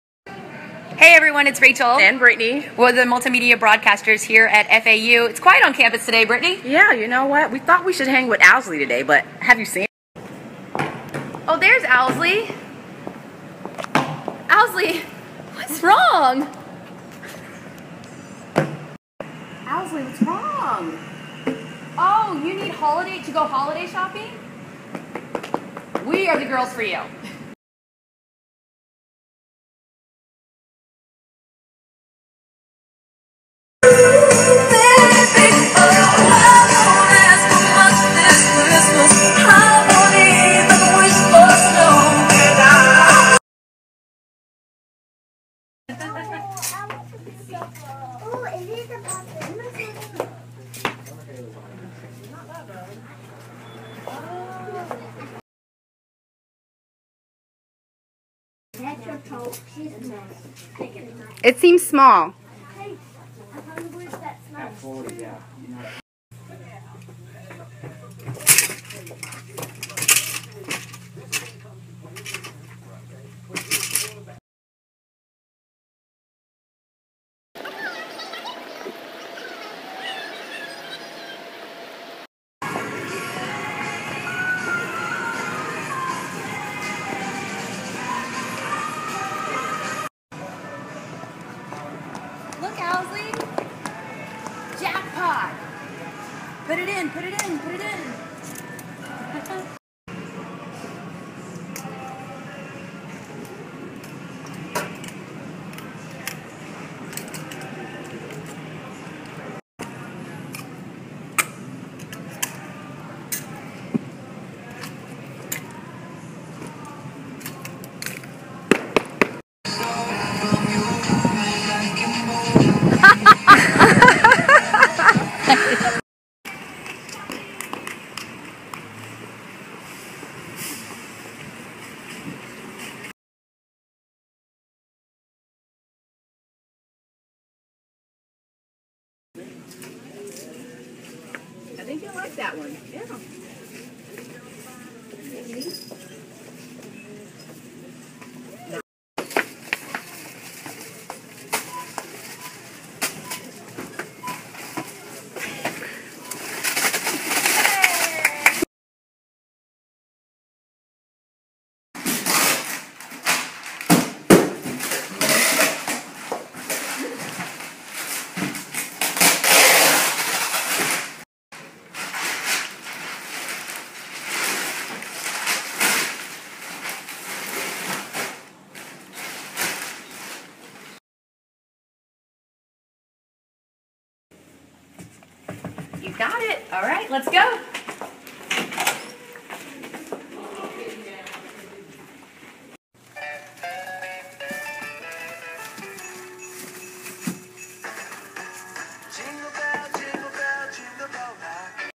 Owlsley Goes Holiday Shopping Snapchat Story
Owlsley waited until the last minute to get holiday gifts, so he enlists some help.